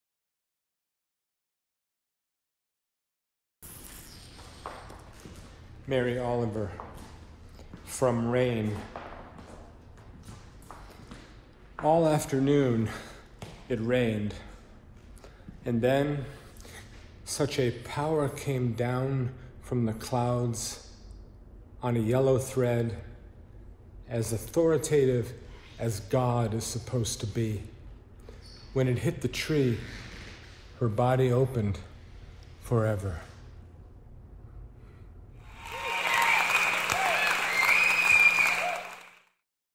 Mary Oliver, (from) Rain, poetry recitation by Brian Paul Allison
From my personal anthology project, and the Art of Collecting Poetry. (Create your own anthology. Stimulate your mind. Enrich your life.) Poetry Theater--tour de force with the Bard of the B---